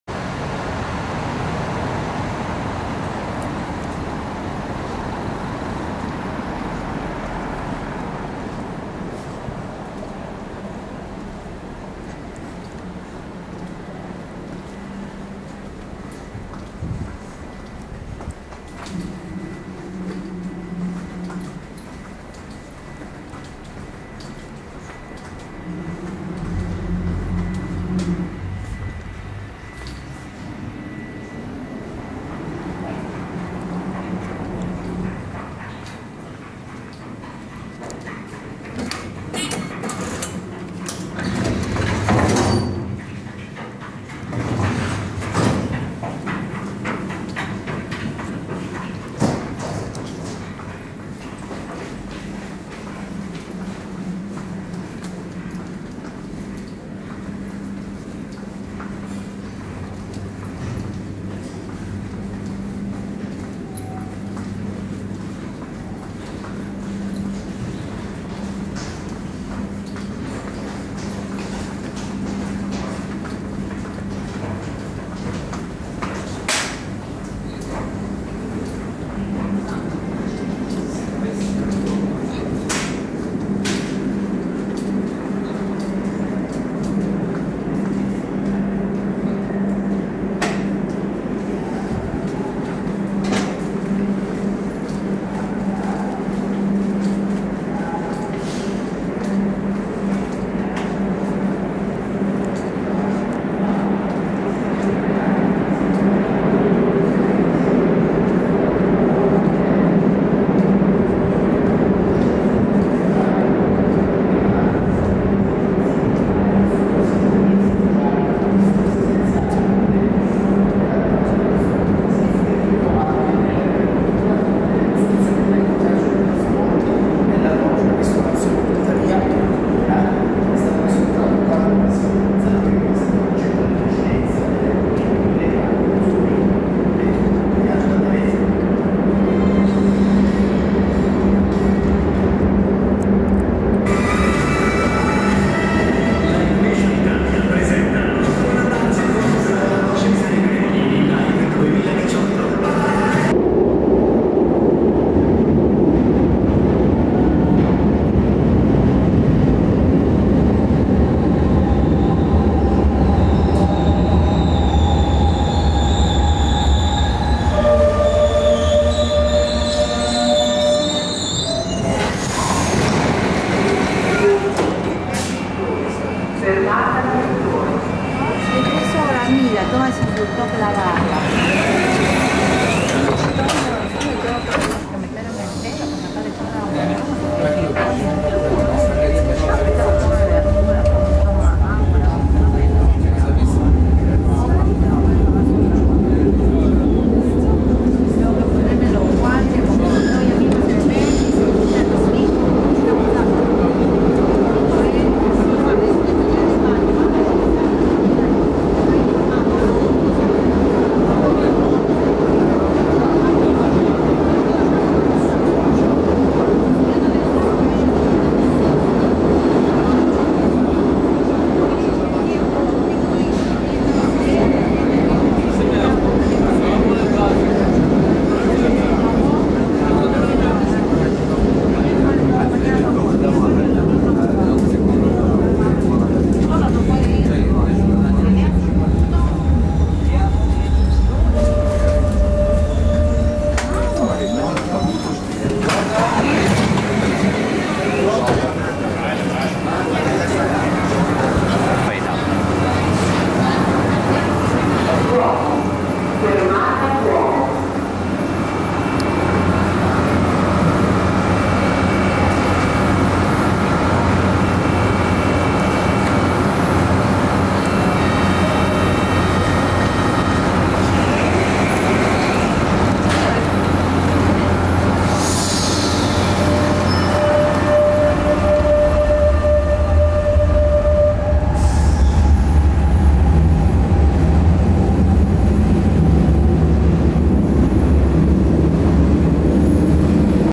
Italy, Milan, Metro night ride from MISSORI to DUOMO
ticket barrier 0:38

metro train 2:30

October 2018